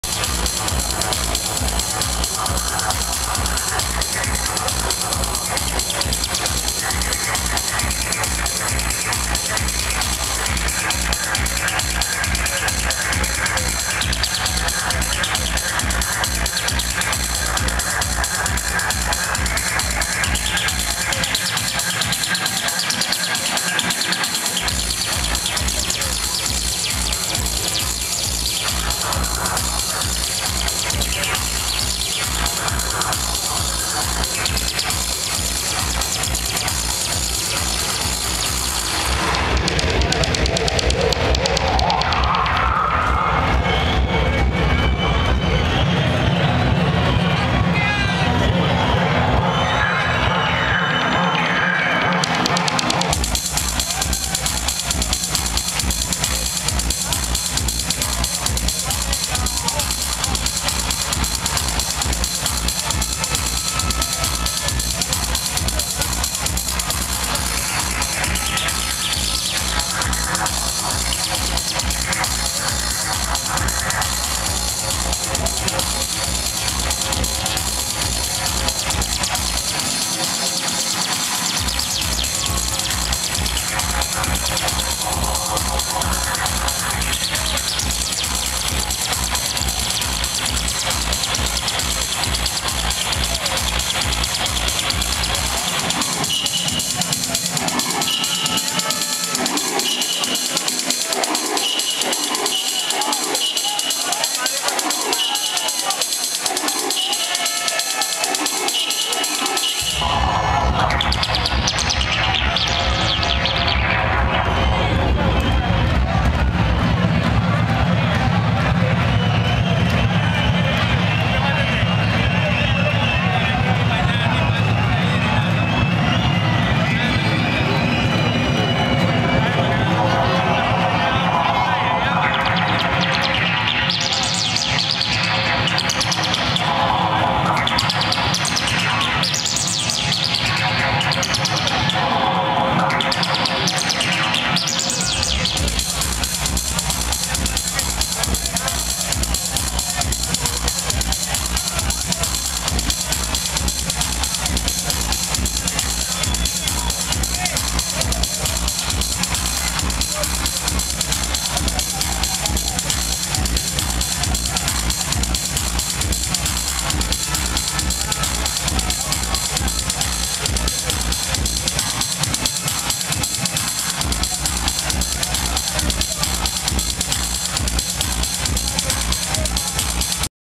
Oscar Mulero. Light & Dark Av set @ Fresh Weekend. La Coruña, Spain (01/07/2011) IV
Oscar Mulero @ Fresh Weekend. La Coruña, Spain (01/07/2011)
"Presents Light and Dark AV set"